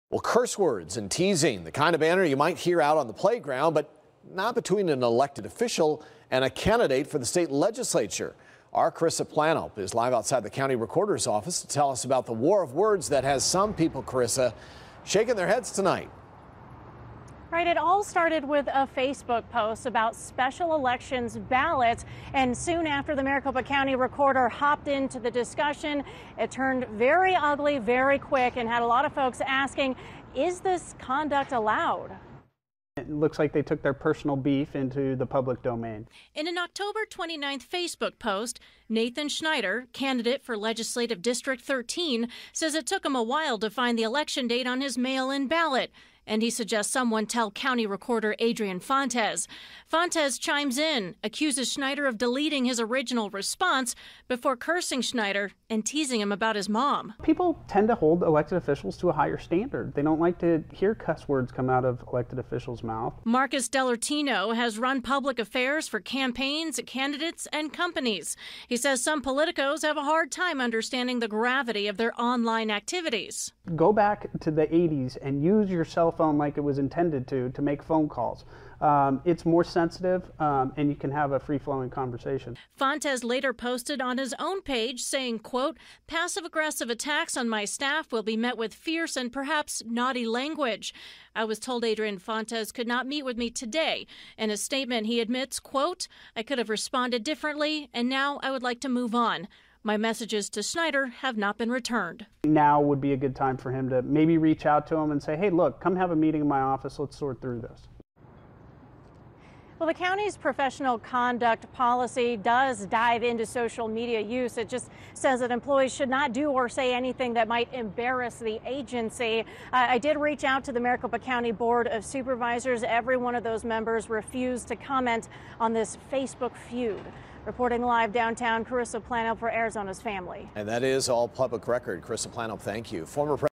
War of words between officials takes ugly turn
Two elected officials took a verbal fight online and it included some conducted some found shocking.